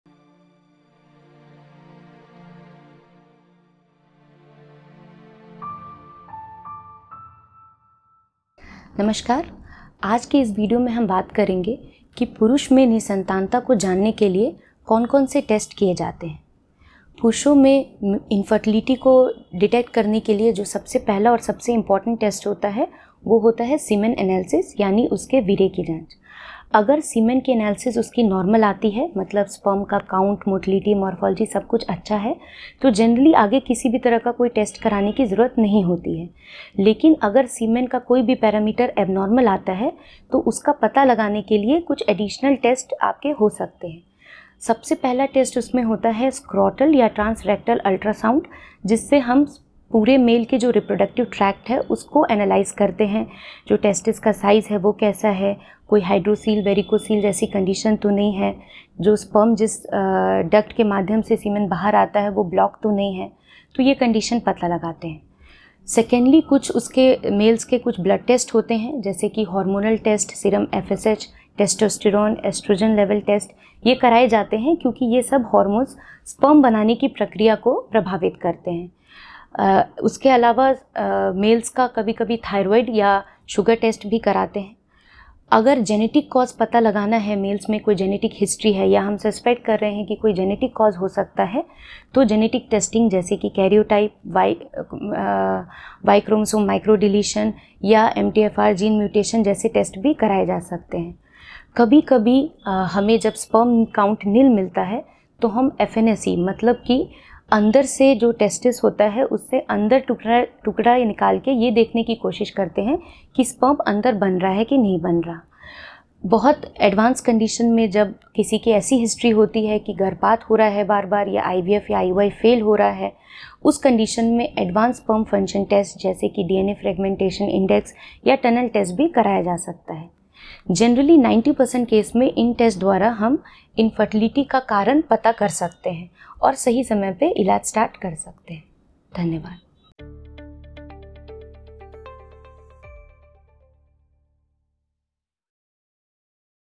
Tests for Male Infertility | Dr Neha Singh | Fertility Specialist | Nova IVF- Gorakhpur
Dr Neha Singh, Fertility Specialist, Nova IVF- Gorakhpur बात करती है-
पुरुष बांझपन के लिए टेस्ट:
पुरुषों में बांझपन का पता लगाने के लिए पहला और सबसे महत्वपूर्ण परीक्षण वीर्य विश्लेषण है। यदि वीर्य विश्लेषण रिपोर्ट सामान्य है, तो यह इंगित करता है कि शुक्राणुओं की संख्या, गतिशीलता और आकारिकी सभी संतोषजनक हैं। इस प्रकार, सामान्य तौर पर, किसी अतिरिक्त परीक्षण की आवश्यकता नहीं होती है।

हालांकि, यदि वीर्य के किसी भी पैरामीटर को असामान्य पाया जाता है, तो कारण का निदान करने के लिए आगे की जांच की जाएगी।

पहला परीक्षण एक अंडकोश या ट्रांसरेक्टल अल्ट्रासाउंड है, जो पूरे पुरुष प्रजनन प्रणाली की जांच करता है और अंडकोष के आकार, स्वास्थ्य और वैरिकाज़- या हाइड्रोसील जैसी स्थितियों की उपस्थिति या अनुपस्थिति सहित कारकों को निर्धारित करता है। जिस नलिका से वीर्य निकलता है वह अवरुद्ध नहीं होती है। तो इन स्थितियों का पता लगाया जाता है।

पुरुषों के लिए कुछ रक्त परीक्षण होते हैं, जैसे हार्मोनल परीक्षण, सीरम एफएसएच, टेस्टोस्टेरोन और एस्ट्रोजन स्तर परीक्षण। ऐसा इसलिए किया जाता है क्योंकि ये सभी हार्मोन स्पर्म बनाने की प्रक्रिया को प्रभावित करते हैं। इसके अलावा कभी-कभी पुरुषों का थायराइड या शुगर टेस्ट भी किया जाता है।

यदि अनुवांशिक कारण का पता लगाना है तो पुरुष में आनुवंशिक इतिहास है या कोई अनुवांशिक कारण हो सकता है। फिर आनुवंशिक परीक्षण जैसे कैरियोटाइप, वाई क्रोमोसोम माइक्रोएलेटमेंट या एमटीएचएफआर जीन म्यूटेशन भी किया जा सकता है।

कभी-कभी जब शुक्राणुओं की संख्या शून्य होती है, तो FNA किया जाता है, जिसका अर्थ है कि अंडकोष के अंदर से एक हिस्सा निकाल लिया जाता है और यह देखने की कोशिश की जाती है कि शुक्राणु अंदर बन रहे हैं या नहीं।

जब कोई स्थिति बहुत उन्नत हो, और किसी व्यक्ति का बार-बार गर्भपात का इतिहास रहा हो, या जब आईवीएफ या आईयूआई सफल नहीं हो रहा हो। एक उन्नत शुक्राणु कार्य परीक्षण, जैसे डीएनए विखंडन सूचकांक या सुरंग परीक्षण, उन परिस्थितियों में भी किया जा सकता है।

आम तौर पर, ये परीक्षण 90% मामलों में बांझपन के अंतर्निहित कारण की पहचान कर सकते हैं, जिससे समय पर प्रारंभिक उपचार की अनुमति मिलती है।

Dr Neha Singh, Fertility Specialist, Nova IVF- Gorakhpur talks about-
Tests for Male Infertility:
The first and most important test for detecting infertility in men is semen analysis.
If the semen analysis report is normal, this indicates that the sperm count, motility, and morphology are all satisfactory. Thus, in general, no additional tests are required.

However, if any of Semen's parameters are found to be abnormal, further testing will be done to diagnose the cause.

The first test is a scrotal or transrectal ultrasound, which examines the entire male reproductive system and determines factors including testes size, health, and the presence or absence of varicose- or hydrocele-like conditions. The duct through which the semen comes out is not blocked. So these conditions are detected.

There are some blood tests for males, such as hormonal tests, serum FSH, testosterone, and estrogen level tests. This is done because all these hormones affect the process of making sperm. Apart from this, sometimes a thyroid or sugar test is also done on males.

If the genetic cause is to be ascertained, there is a genetic history in the male or that there may be a genetic cause. Then genetic testing such as karyotype, Y chromosome microdeletion or mthfr gene mutation can also be done.

Sometimes when a sperm count is nil, FNA is done, which means a part is taken out from the inside of the testes and tried to see whether the sperm is being made inside or not.

When a condition is very advanced, and a person has a history of repeated abortions, or when IVF or IUI is not succeeding. An advanced sperm function test, such as a DNA fragmentation index or tunnel test, can also be performed in those circumstances.

Generally, these tests can identify the underlying reason of infertility in 90% of instances, allowing for timely initial treatment.

00:00  Introduction
00:17  Semen analysis
00:45  Transrectal Ultrasound
01:06  Blood Tests
01:26  Genetic Cause
01:56  Advanced Sperm Function Test